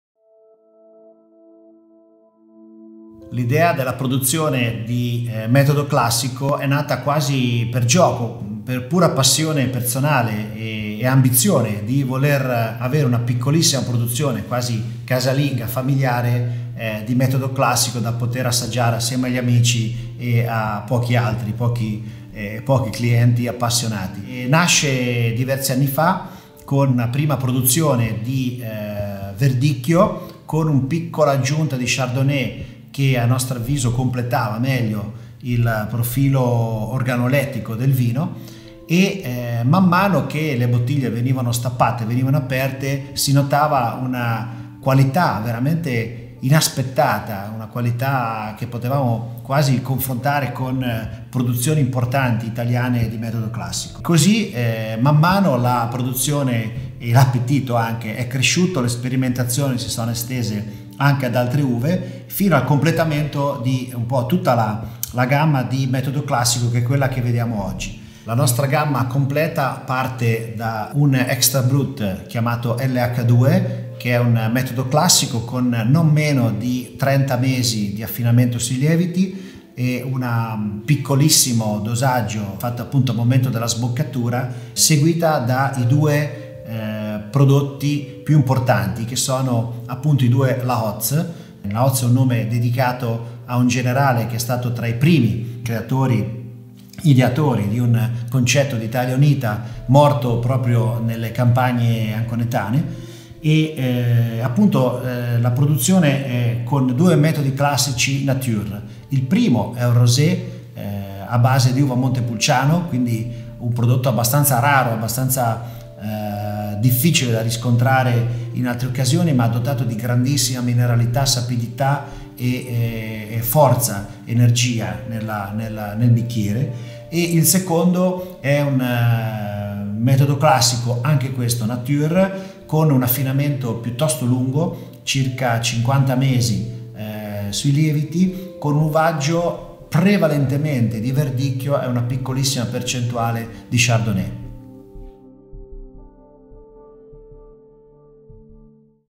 Michele Bernetti racconta la linea di Metodo Classico
Una piccolissima produzione “privata” nata dall’interesse e dalla curiosità personale di Michele Bernetti diventa una produzione di successo. LH2, La Hoz e La Hoz Rosé Nature: una collezione a tiratura limitata che si pone tra le vette del panorama del Metodo Classico nazionale.